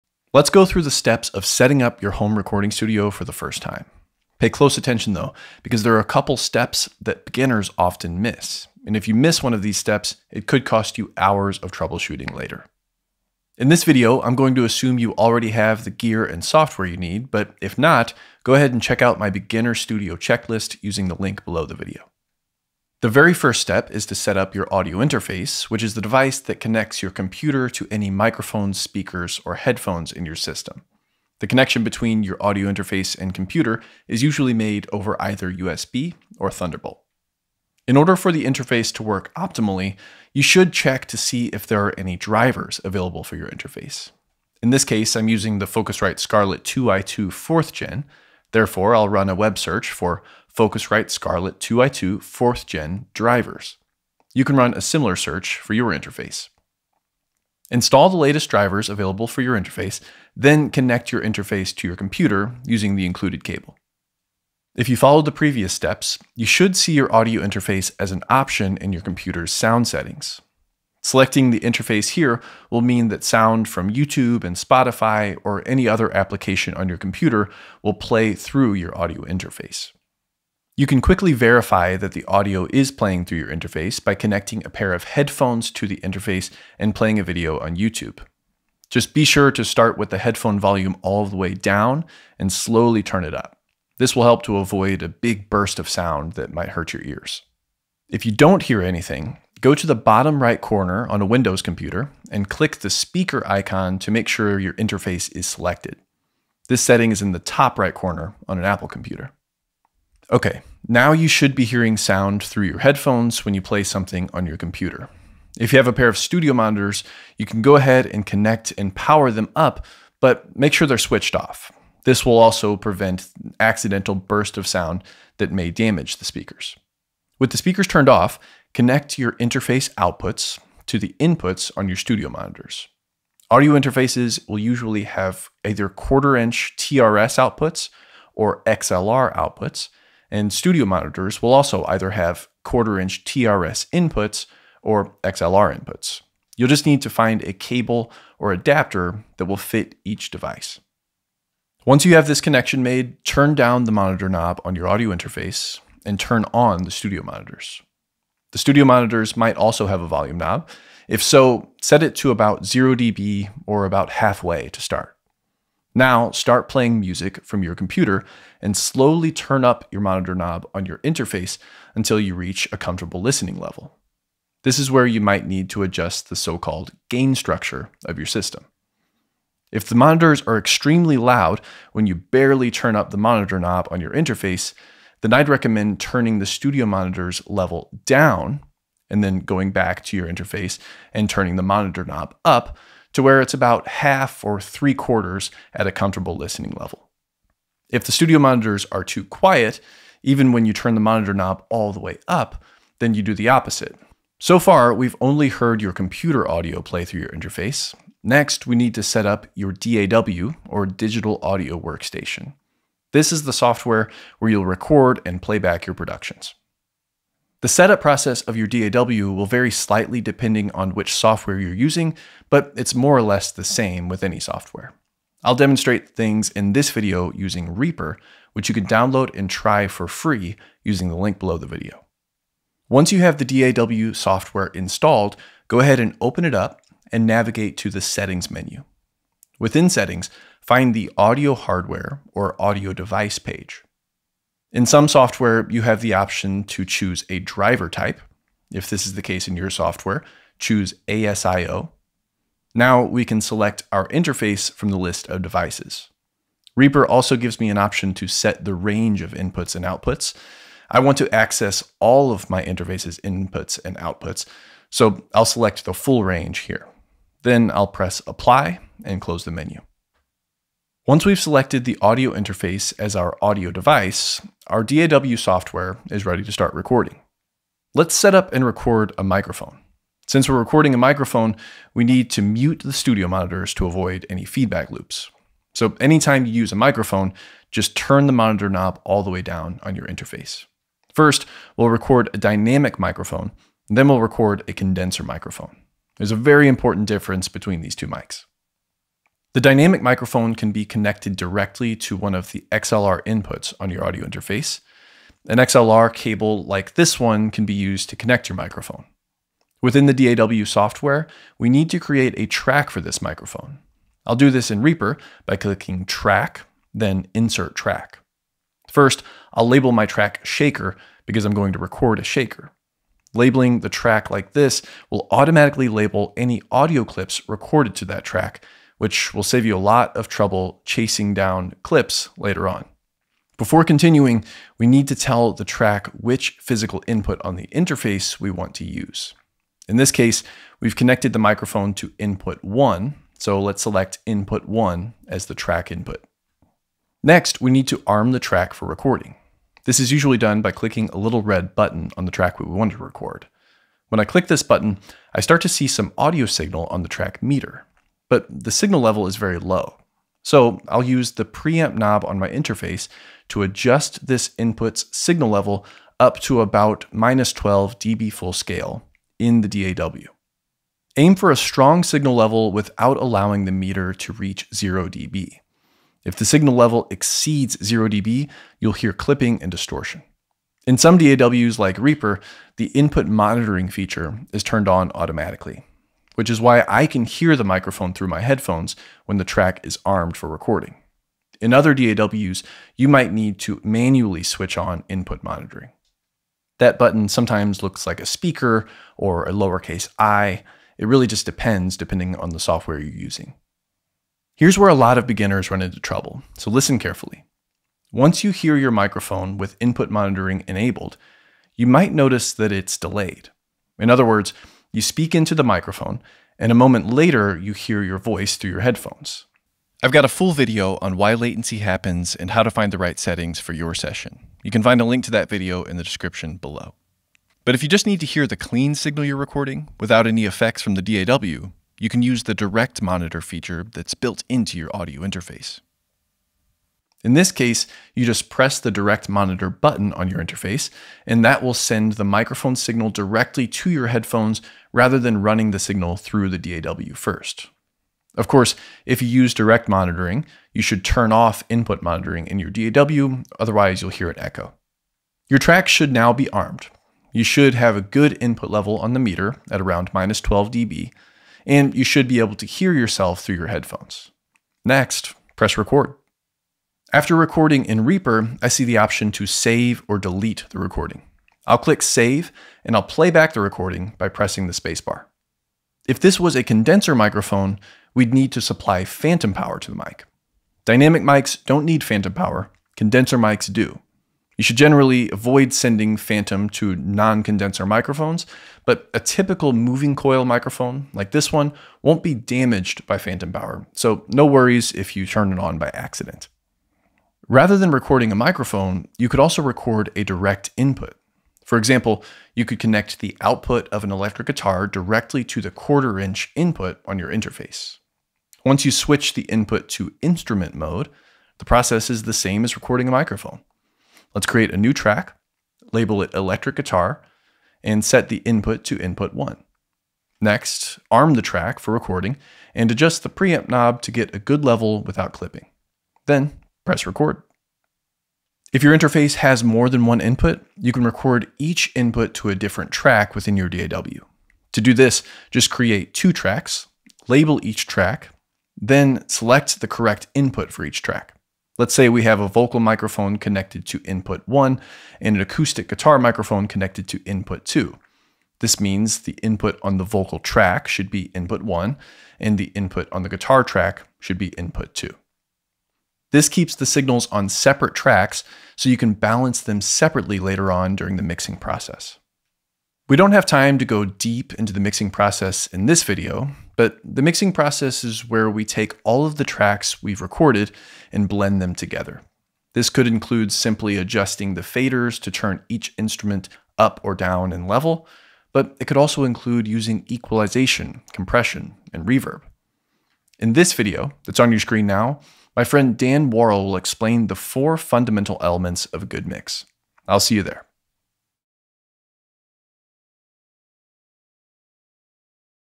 Step-by-Step Tutorial: Setting Up Your First Audio Interface for Home Recording
Watch this video if you're setting up a home studio for the first time. These steps will help you avoid the most common mistakes. Learn how to setup an audio interface, a digital audio workstation (DAW), and start your recording session.

+ Focusrite Scarlett 2i2 4th Gen (Best Deals)
+ Reaper (DAW)

00:00 - Beginner Home Studio Checklist
00:23 - STEP 1: Audio Interface
01:21 - STEP 2: Phones & Speakers
03:24 - STEP 3: DAW
04:37 - STEP 4: Microphones
05:09 - Setting Up A Track
06:24 - Input Monitoring
07:09 - Direct Monitoring
07:55 - Phantom Power (+48V)
08:17 - Direct Input (Instrument)
08:48 - Multiple Tracks
09:23 - STEP 5: Mixing